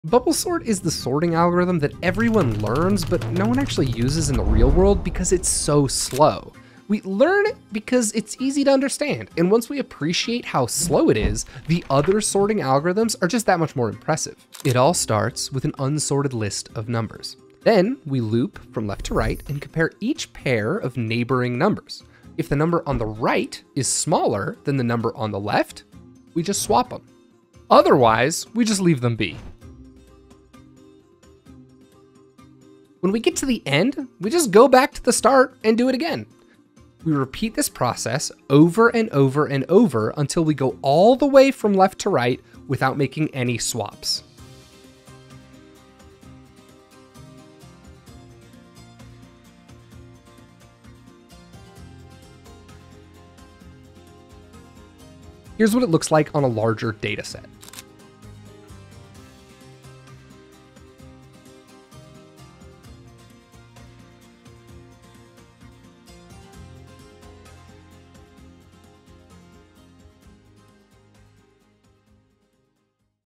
How does Bubble Sort work?
Now what's this talk about bubbles on a programming video? We certainly aren't goofing around are we? Well, bubble sort is probably one of the first algorithms we learn, and also one of the slowest, but it introduces us to the wonderful world of sorting algorithms, where down the line we can further explore the complexity and speed of sorting.